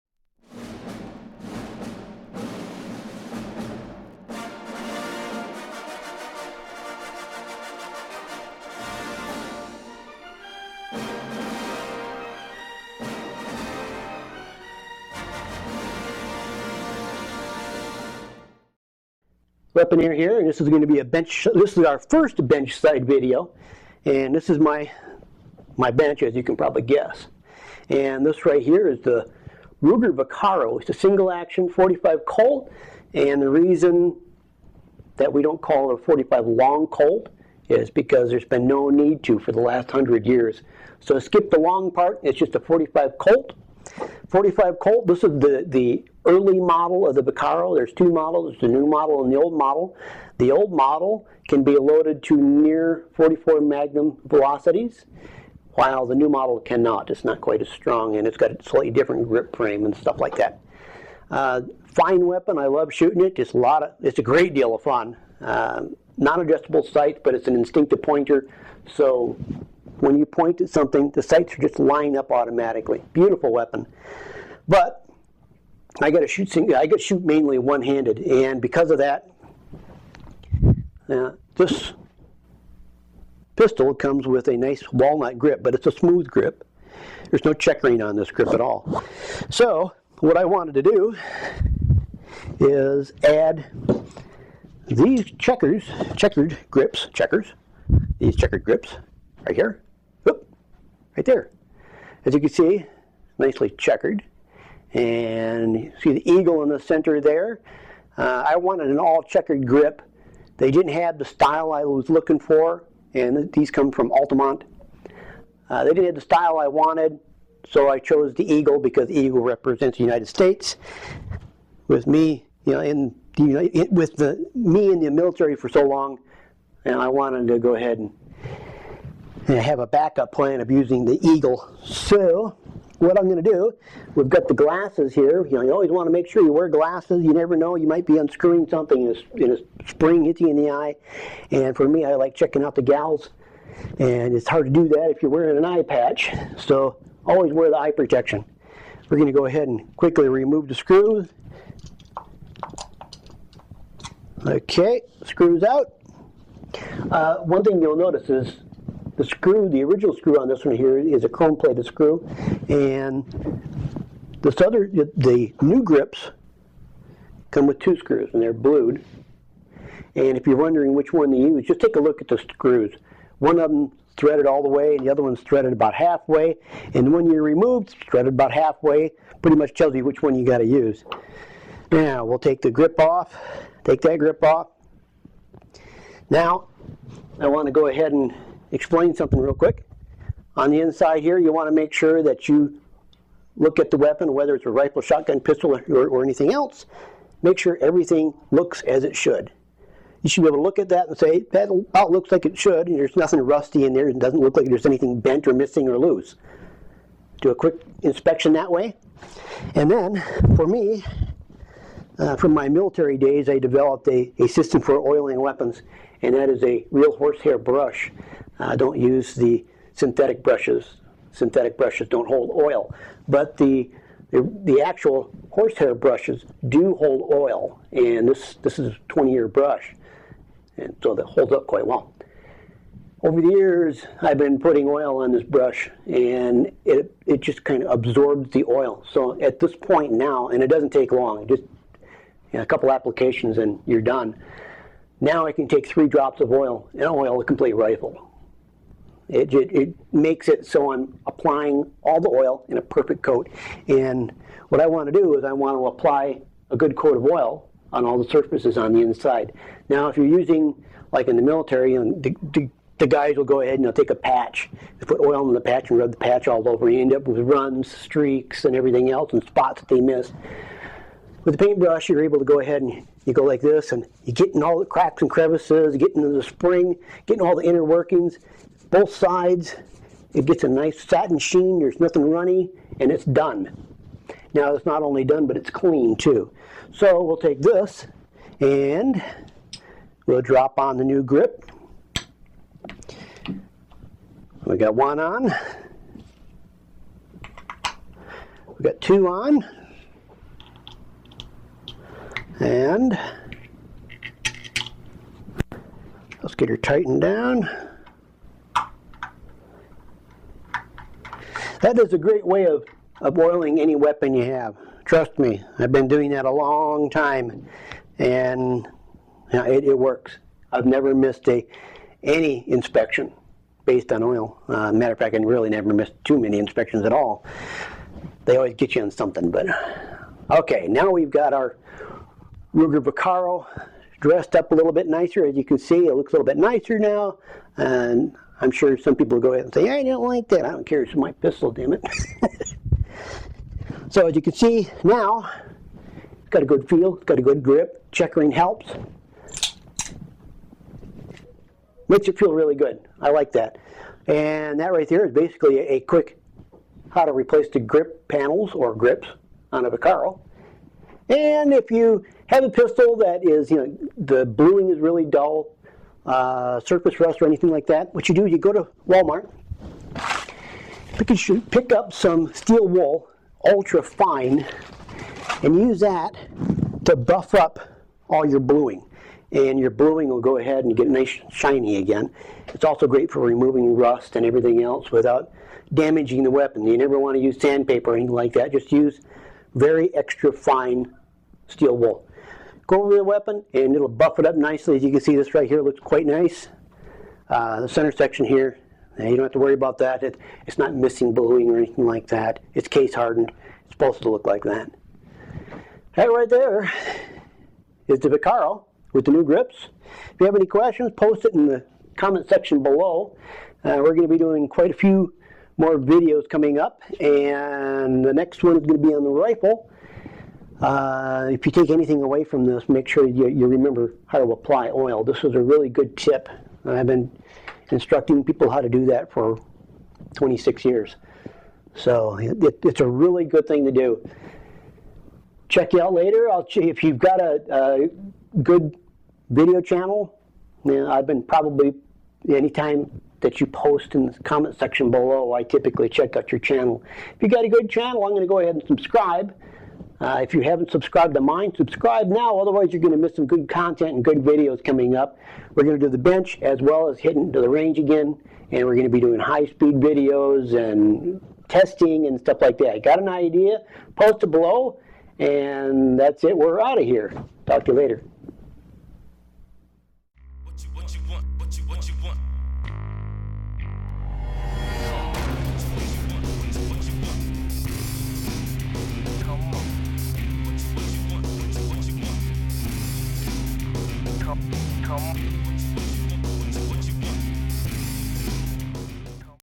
Changing grips on the Ruger Vaquero 45 Colt Single Action Pistol
Just a video of me Changing grips on the Ruger Vaquero 45 Colt Single Action, and providing a few tips along the way for anyone interested in working on pistols.

it would seem that YouTube hosed the audio on one of the channels  ;(